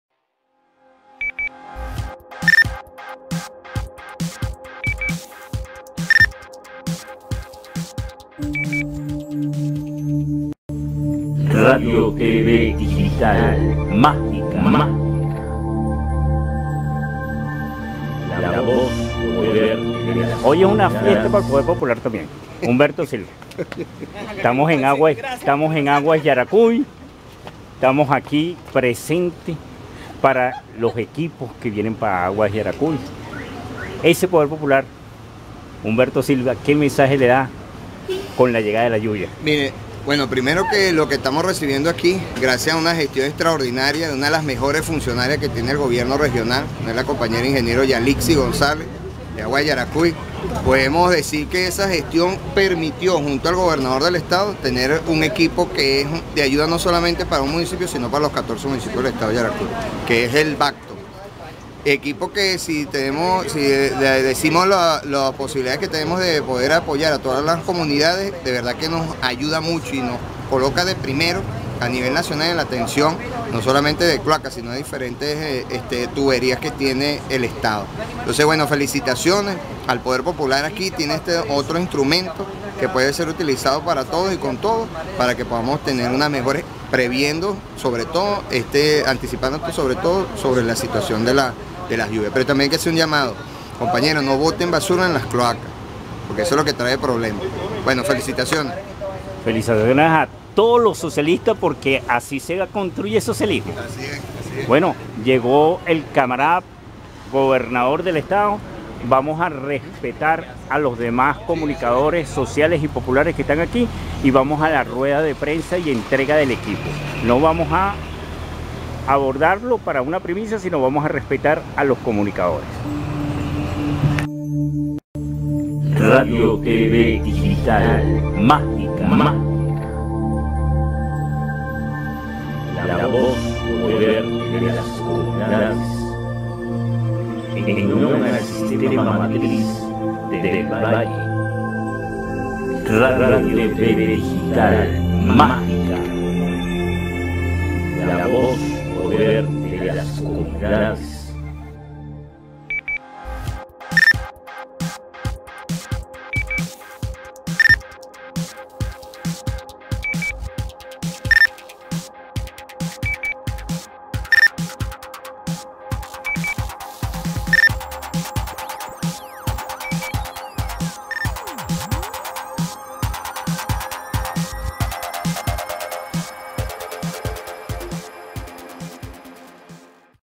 EsNoticiaHoy Humberto Silva, Dirección del Poder Comunal del Estado Yaracuy
Producciones de La Fundación Comando Comunicacional Hugo Chavez
Programas, Tip, Publicidad, Actividades en General que Generen La Noticia Veraz y Auntentica, Siendo Irreverente como Pregoneros de la Calle...!!!